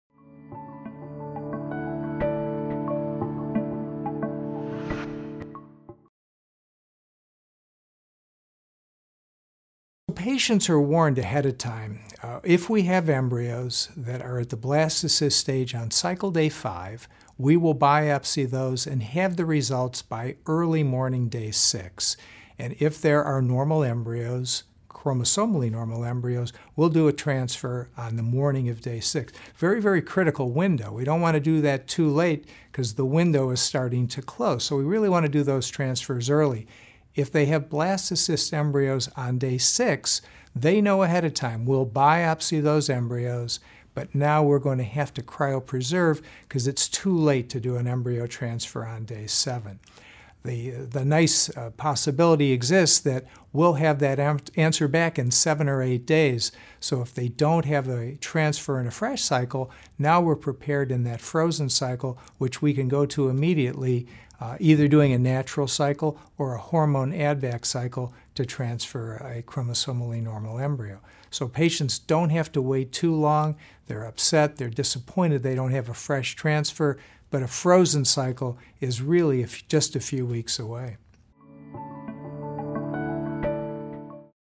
Comprehensive Chromosomal Screening (CCS) in IVF:
Martin Freedman, MD RMA of Philadelphia

[Text on Screen]: Fertility Authority, Your Most Trusted Source

Ask The Experts

 How is comprehensive chromosomal screening (CCS) incorporated into an IVF cycle?

Martin Freedman, MD, RMA of Philadelphia: Patients are warned ahead of time if we have embryos that are at the blastocyst stage on cycle Day 5, we will biopsy those and have the results by early morning Day 6. And if there are normal embryos, chromosomally normal embryos, we’ll do a transfer on the morning of Day 6.

Very, very critical window. We don’t want to do that too late, because the window is starting to close. So, we really want to do those transfers early.

If they have blastocyst embryos on Day 6, they know ahead of time, we’ll biopsy those embryos, but now we’re going to have to cryopreserve, because it’s to late to do an embryo transfer on Day 7.

The nice possibility exists that we’ll have that answer back in seven or eight days. So, if they don’t have a transfer in the fresh cycle, now we’re prepared in that frozen cycle, which we can go to immediately, either doing a natural cycle or a hormone add-back cycle to transfer a chromosomally normal embryo.

So, patients don’t have to wait too long. They’re upset. They’re disappointed, They don’t have a fresh transfer. But a frozen cycle is really just a few weeks away.